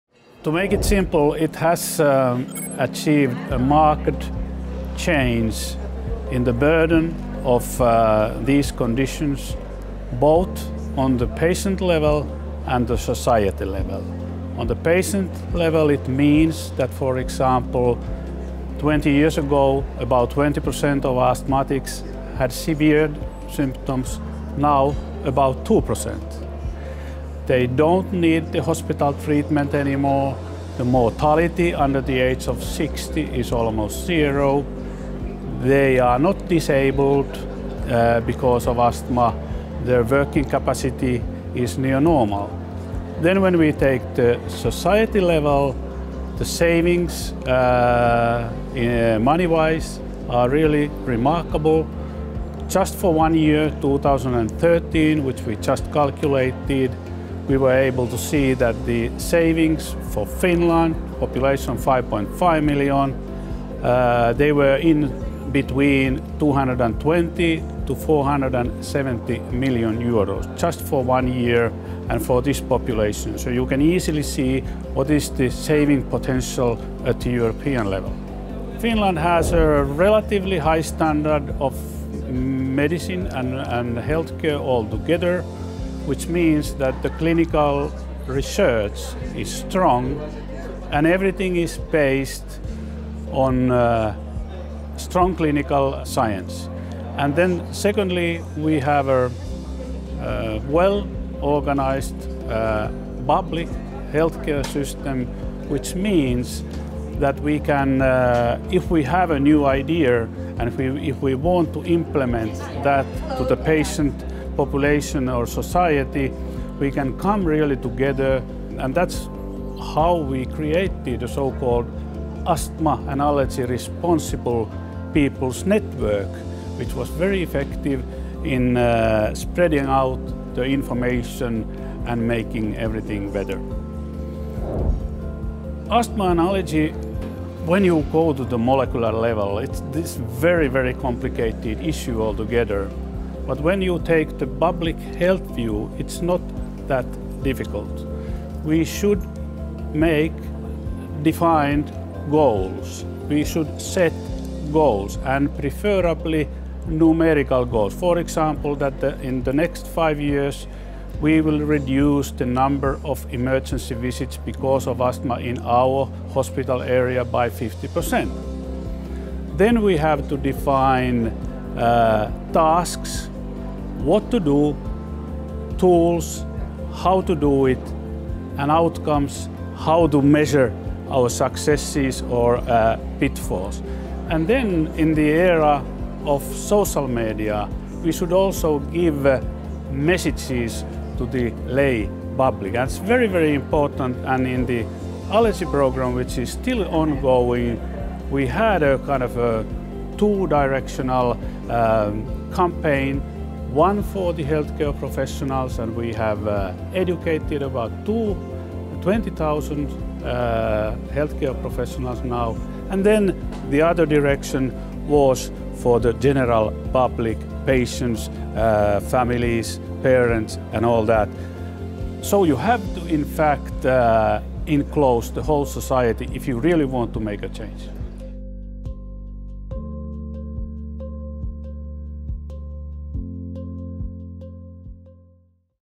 Finnish Allergy and Asthma Programme | Prof. Tari Haahtela
Find out what Prof. Tari Haahtela has to say about the Finnish Allergy and Asthma Programme.

0:00 What has the Finnish Allergy and Asthma programme achieved?
1:20 What have been the critical success factors of this programme?
2:15 What is the most important lesson that the EU can learn from your approach?